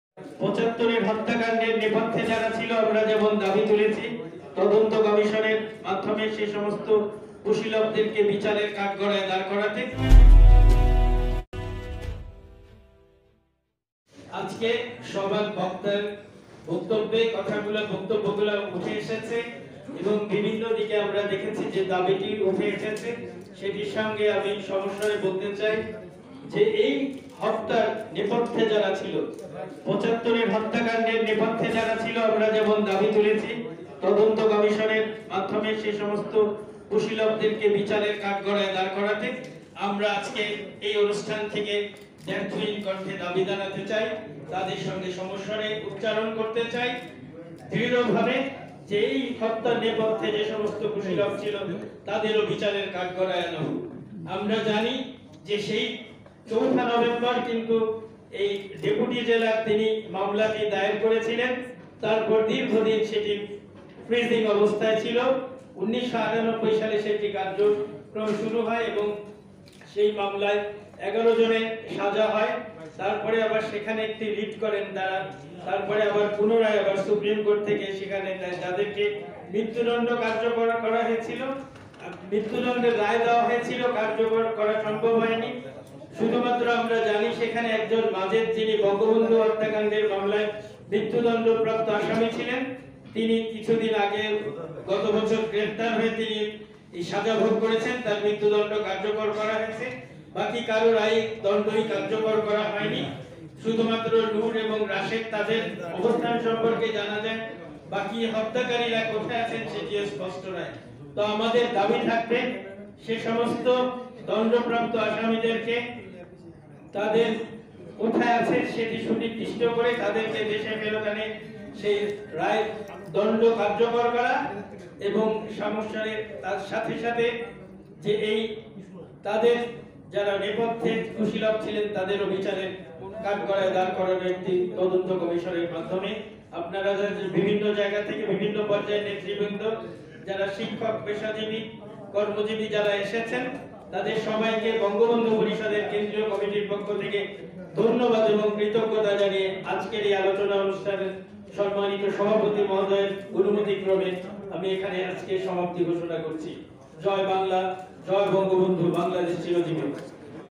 ময়মনসিংহ বিভাগের স্বাশিপের বিভাগীয় প্রতিনিধি সভা -২০২১
অপরাধী সে যে দলেরই হোক না কেন তাকে আইনের আওতায় আনা: উপাচার্য অধ্যাপক ড. আক্তারুজ্জামান

৩রা নভেম্বর ঐতিহাসিক জেল হত্যা দিবস স্মরণে বঙ্গবন্ধু পরিষদ আয়োজিত সিরডাপ মিলনায়তনে আজকের আলোচনা সভা। বঙ্গবন্ধু পরিষদের সভাপতি মন্ডলীর সদস্য  ঢাকা বিশ্ববিদ্যালয়ের মাননীয় উপাচার্য অধ্যাপক ড. আক্তারুজ্জামান এর সভাপতিত্বে বঙ্গবন্ধু পরিষদের সভাপতি মন্ডলীর সদস্যগণ আলোচনায় অংশ নেন।